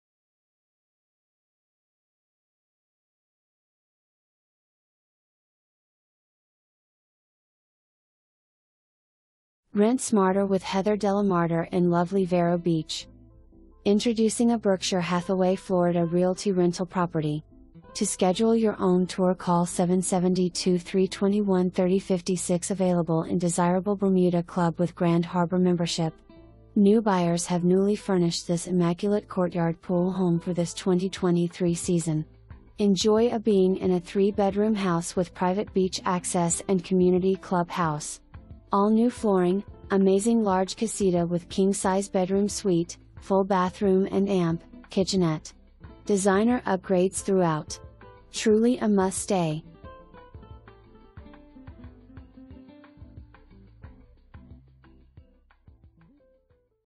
9025 Englewood Court
Available in Desirable Bermuda Club with Grand Harbor Membership! New Buyers have Newly Furnished this immaculate Courtyard Pool Home for this 2023 Season. Enjoy a Being in a 3 Bedroom House with Private beach access and Community Club house. All newFlooring, Amazing Large Casita with King Size Bedroom Suite, Full bathroom &amp; Kitchenette. Designer upgrades throughout. Truly a must stay!

Heather Delamarter
Berkshire Hathaway HomeServices Florida Realty